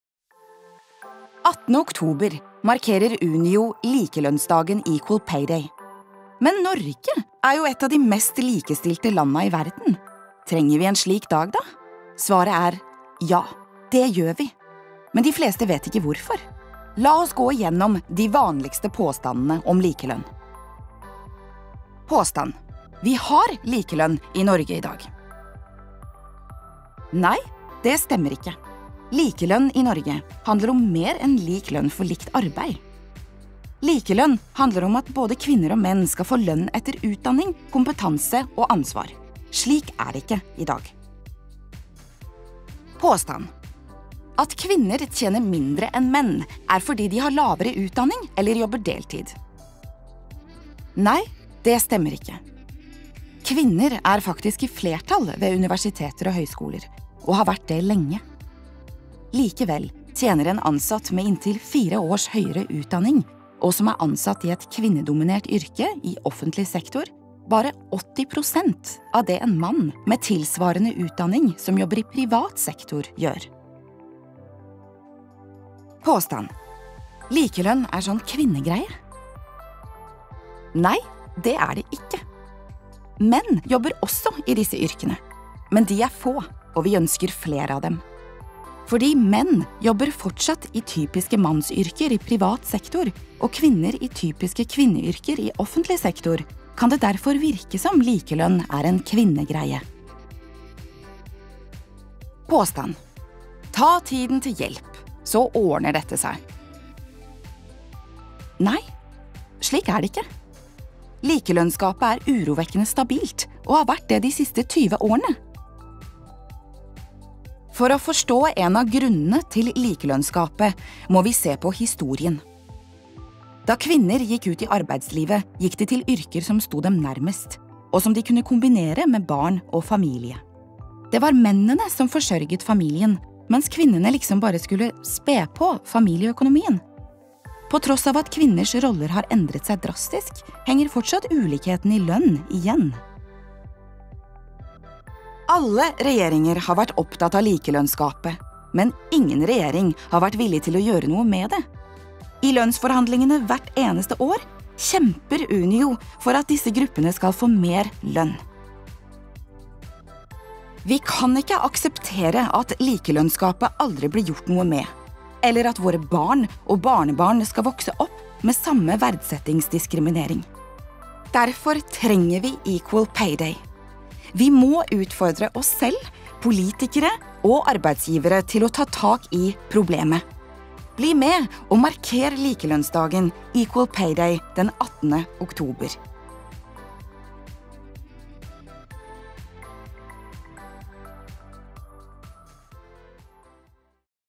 Likelønnsdagen 2017
Den 18. oktober vil Unio markere Likelønnsdagen (Equal Pay Day). Denne filmen viser hvorfor det fortsatt er nødvendig. Filmen er laget i samarbeid med Zoaring.